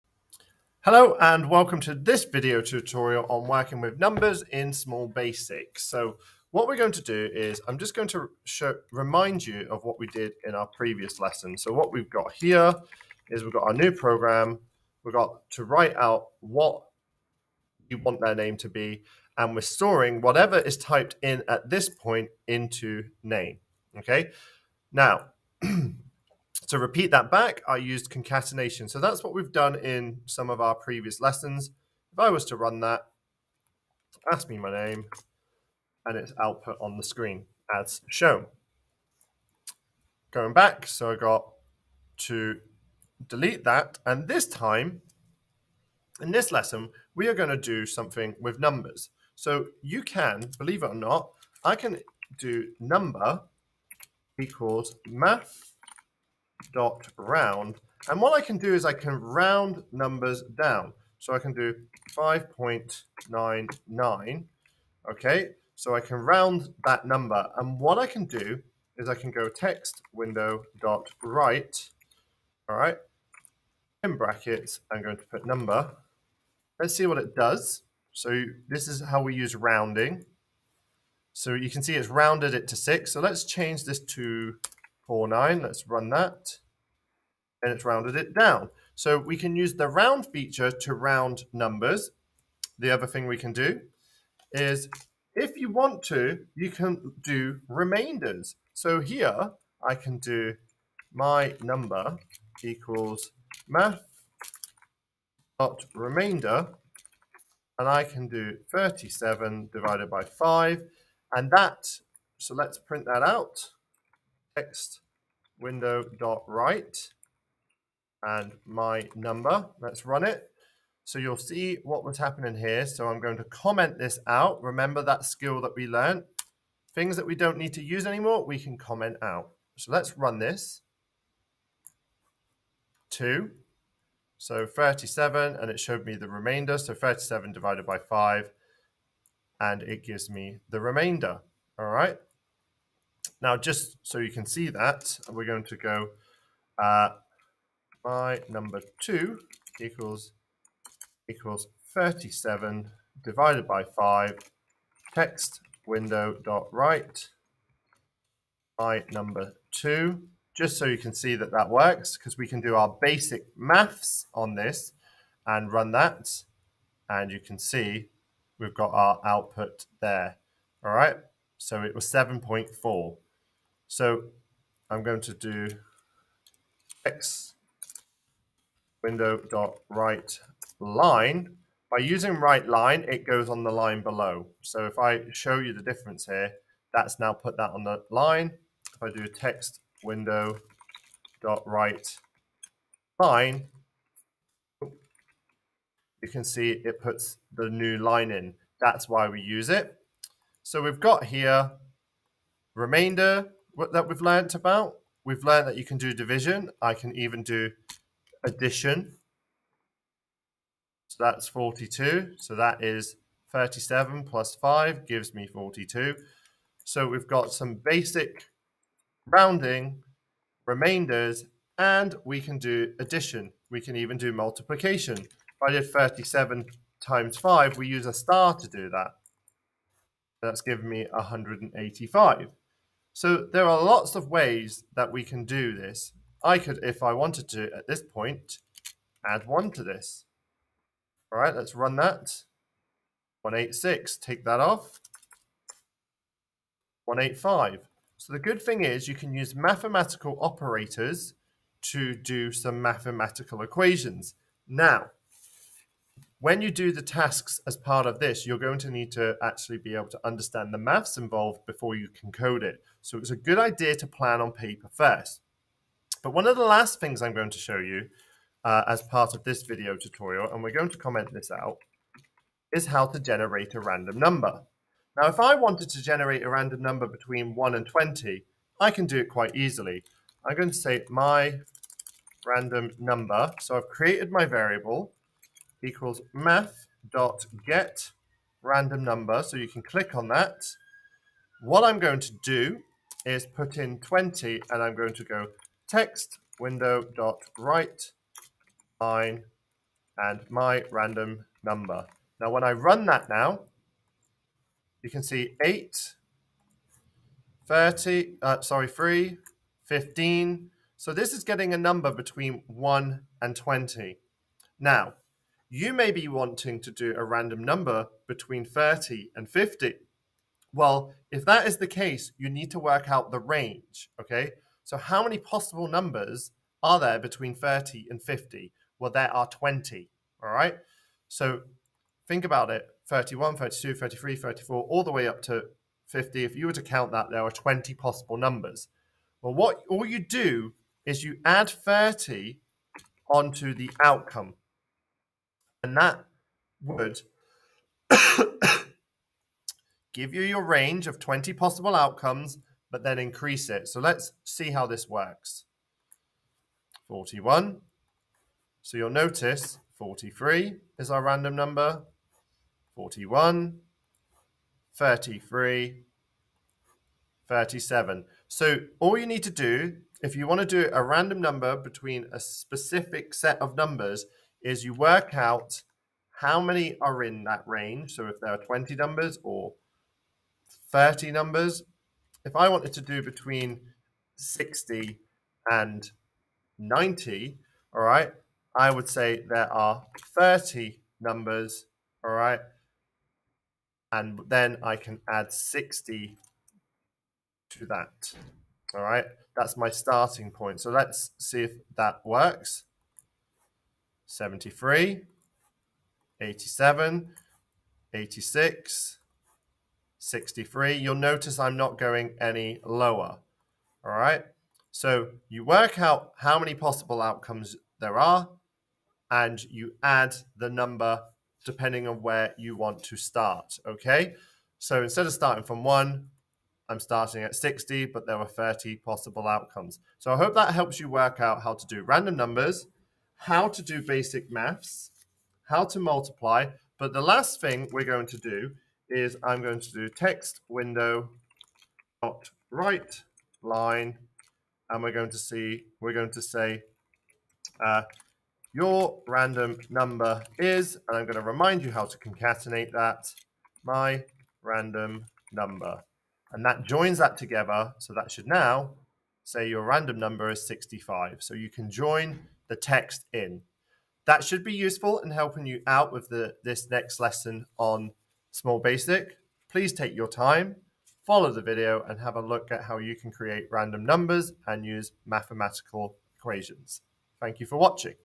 Lesson 4 - Using Small Basic - Using Math / Maths - Random Numbers - Rounding
This video will show you how to use Maths within Small basic. It'll help you program using basic operators such as + - * / and you'll also be able to randomise numbers. You should also be able to round numbers at the end of this tutorial.